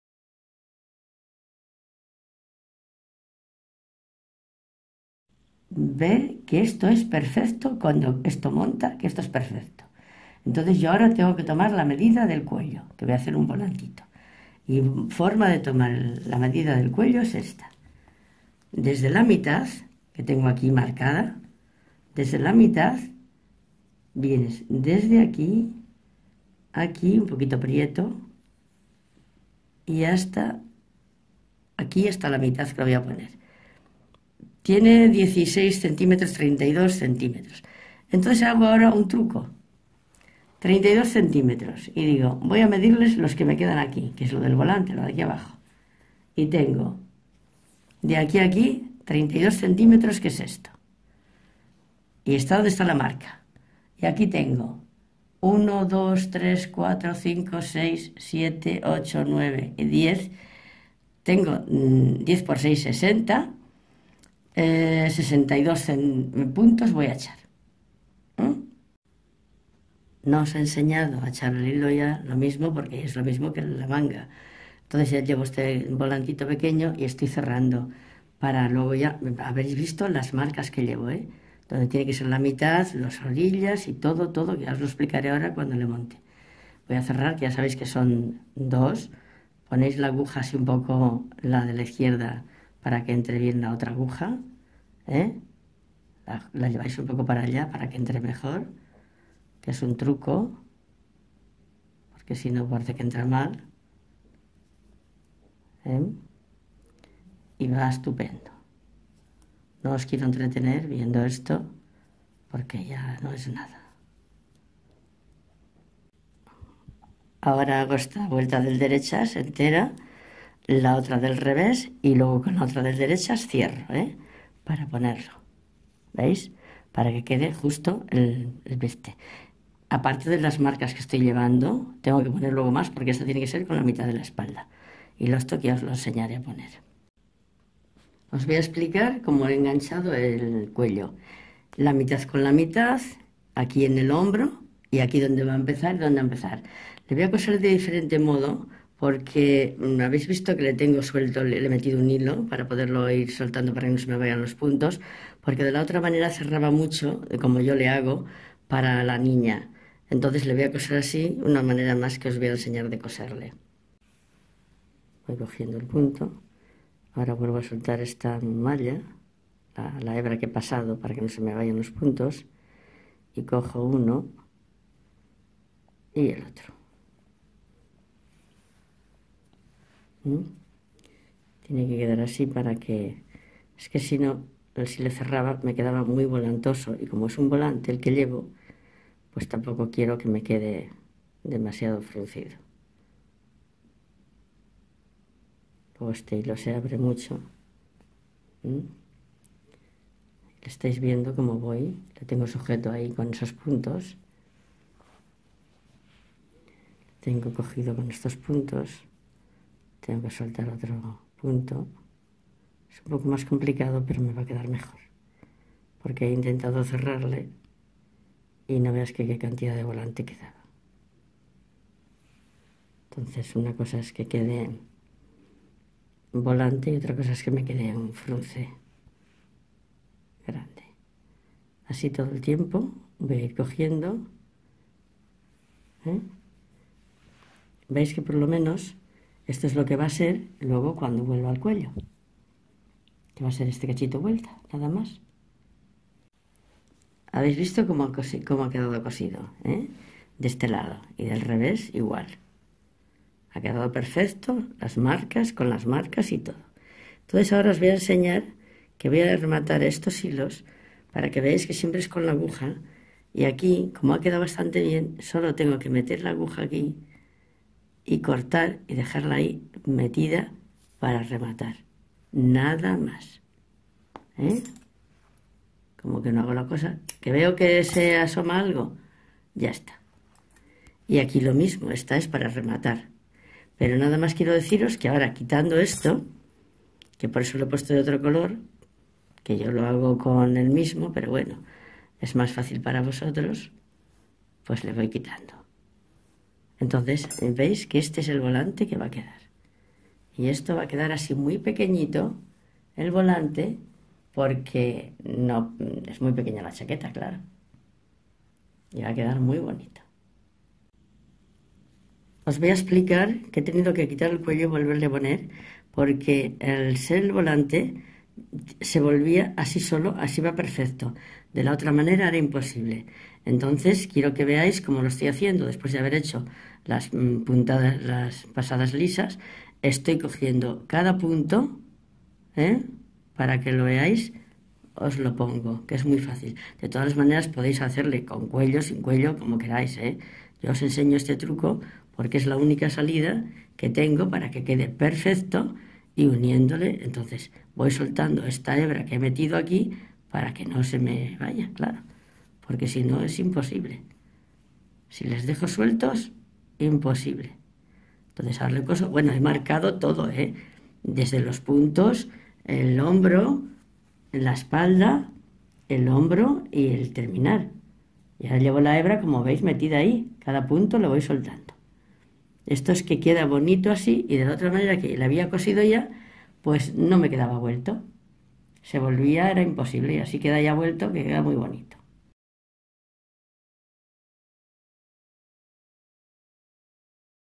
Cómo hacer un cuello para chaqueta de niña
En este vídeo verás cómo tomar medidas para hacer el cuello y cómo coserlo!!!  Llegamos al final!!! Os quedará una obra preciosa!!